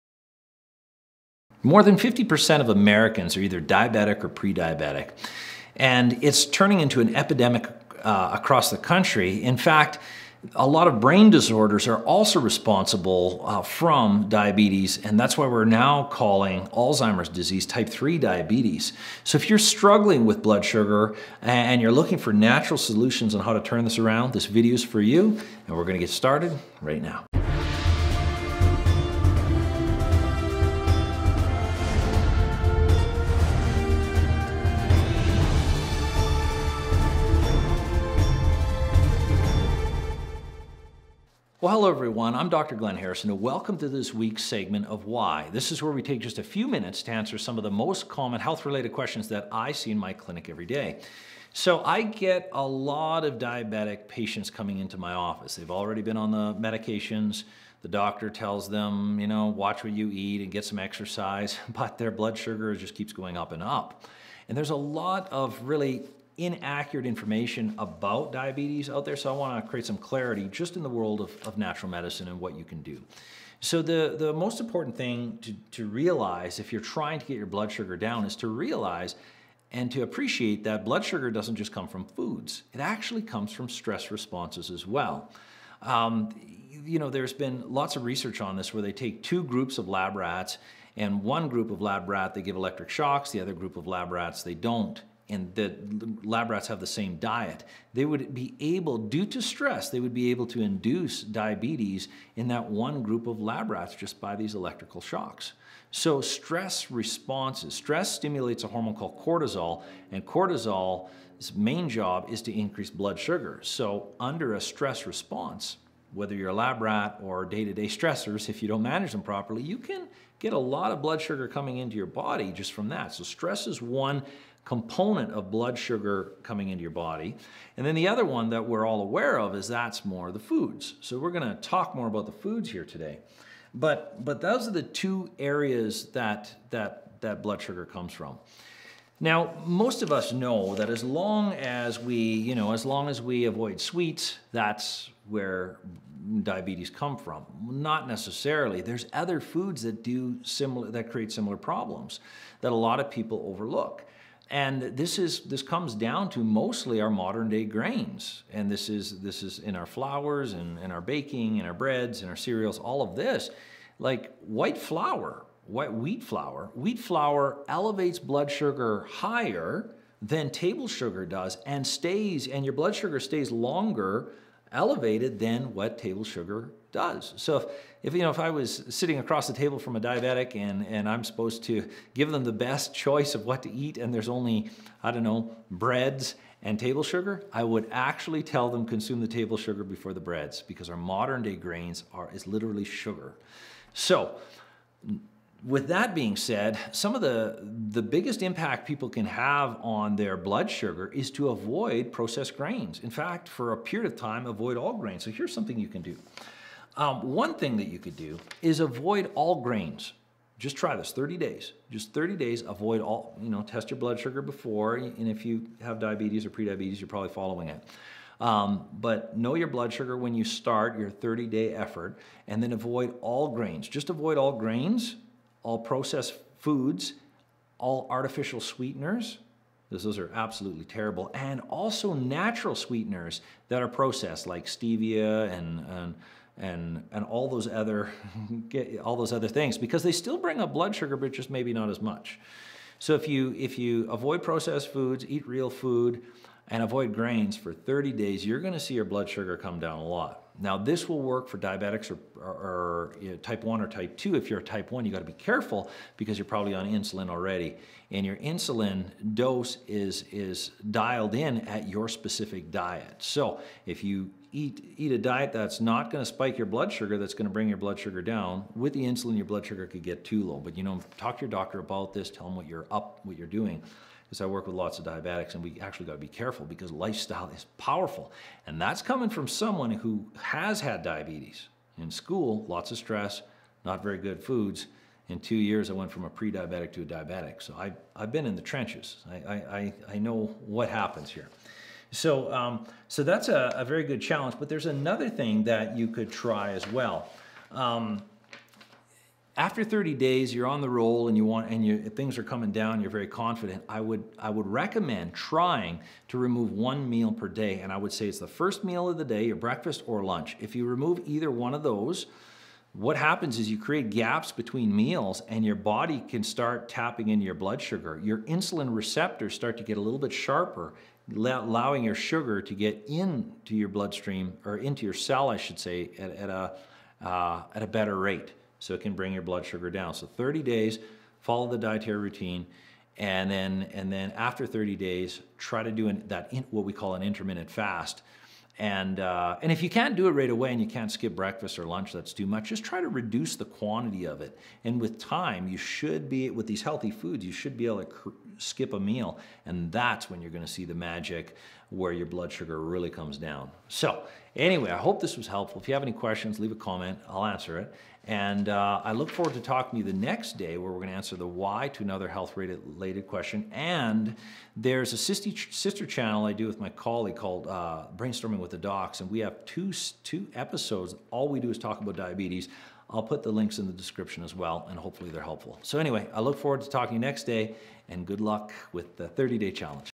Lower Blood Sugar in 30 Days | Natural And Effective
Learn how you can lower your blood sugar in 30 days naturally with lifestyle medicine
00:00 Introduction
01:21 1- Where Blood sugar comes from
01:49 2- Purpose of Cortisol
02:46 3- Wheat Flours Effect On Blood Sugar
03:27 4-Dietary Strategies
04:10 5-Effects on Diabetes
05:03 6-Removing a meal
05:17 7- Insulin Receptors
06:07 8-Brainstorming with the docs